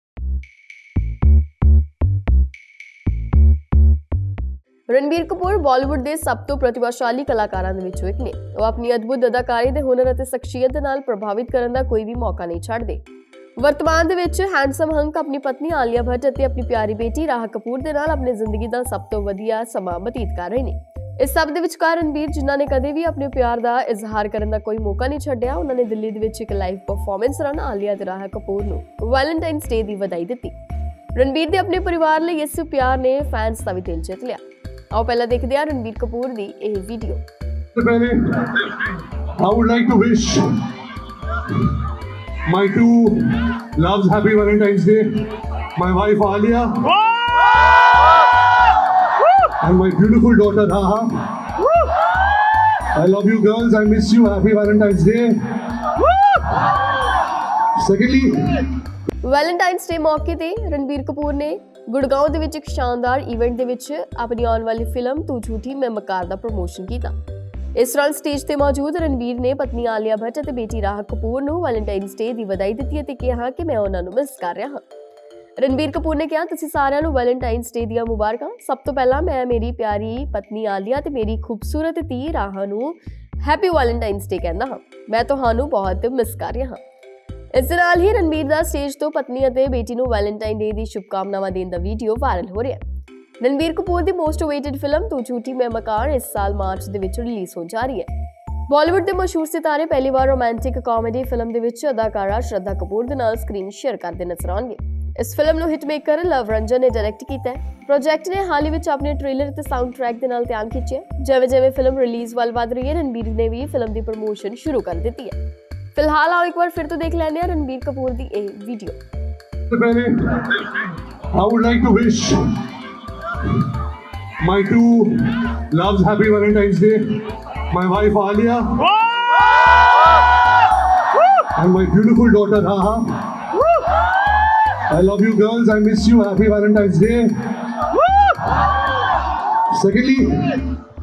ਲਾਈਵ ਕੰਸਰਟ 'ਚ Ranbir ਨੇ Alia ਤੇ ਬੇਟੀ Raha ਨੂੰ ਕੀਤਾ ਯਾਦ, ਵੈਲੇਨਟਾਈਨ ਡੇ ਦੀ ਦਿੱਤੀ ਵਧਾਈ
Ranbir Kapoor First Time Talks About Daughter Raha, Misses Her During TJMM Promotions
Ranbir Kapoor, Daughter Raha, Alia Bhatt, Bollywood Actor, Bollywood Update